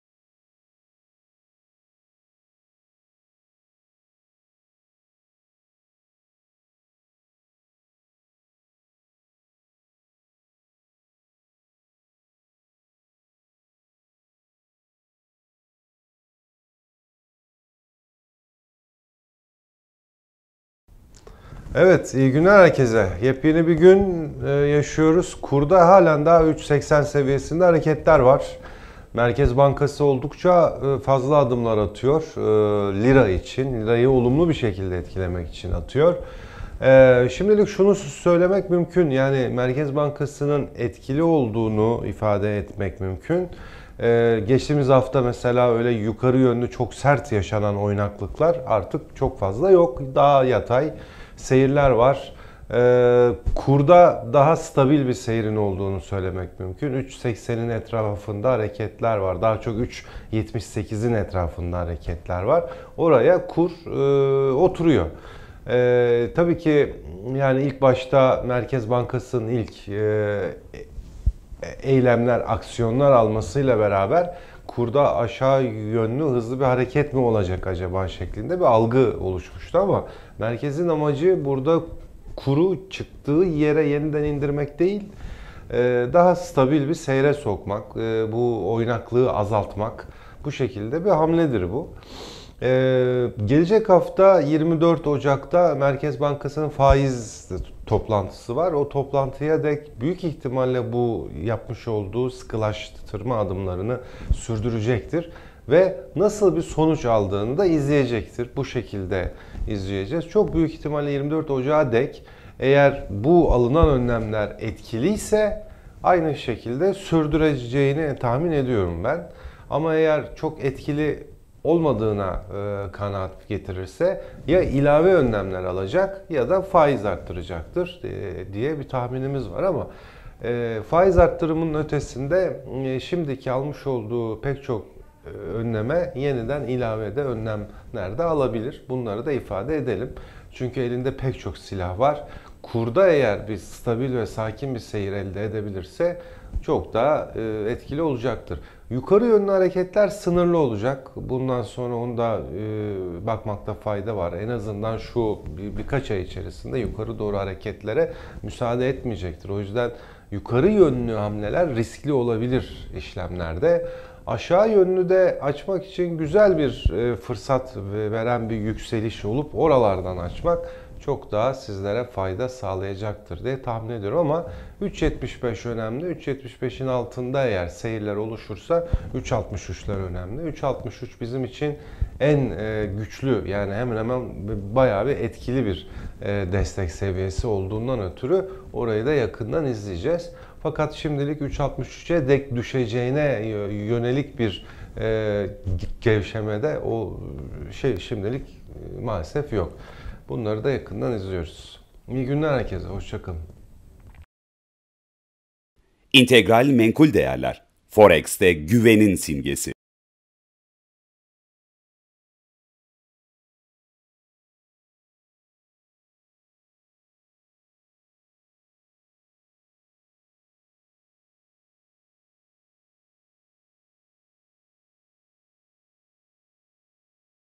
19.01.2017 Piyasa Gündemi
İntegral Menkul Değerler Araştırma Direktörü Tuncay Turşucu sizin için piyasa gündemini aktarıyor.